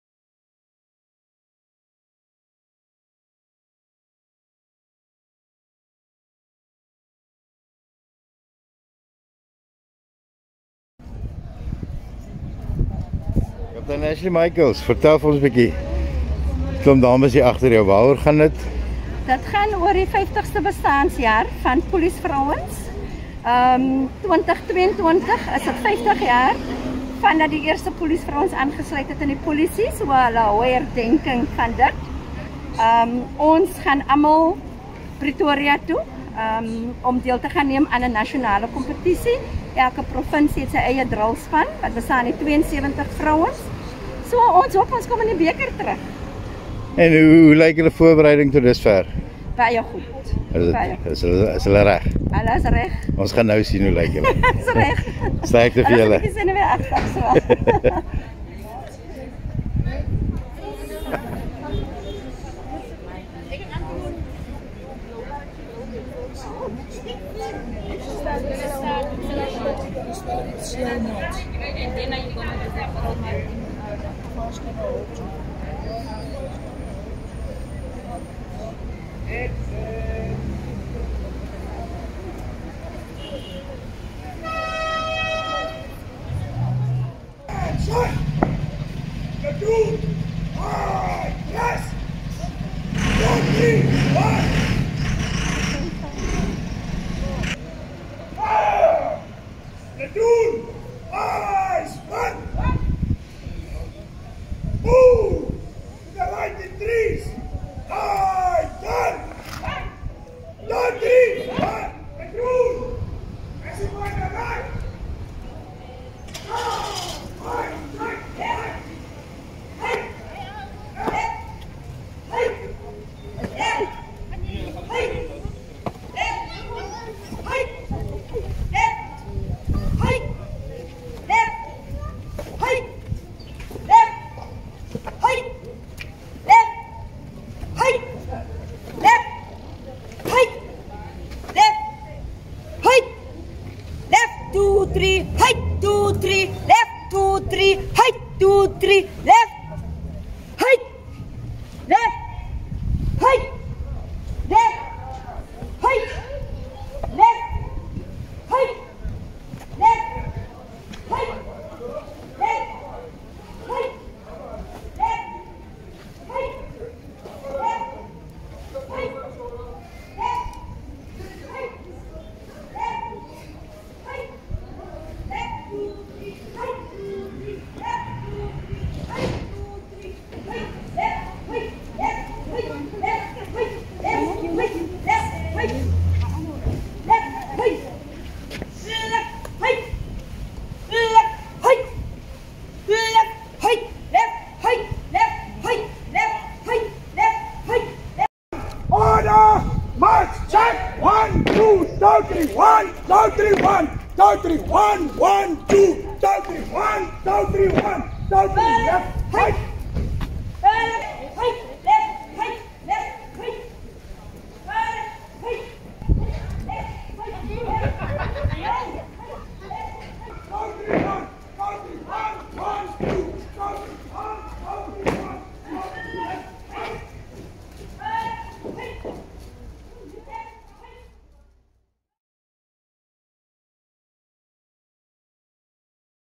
Women's drill squad exhibit their skills
The Western Province women's drill squad performed a drill exhibition through the Mossel Bay town centre today, 21 January. The squad of 72 marched down Marsh Street to the Point with lots of public cheering them.
Video: Jannie du Plessis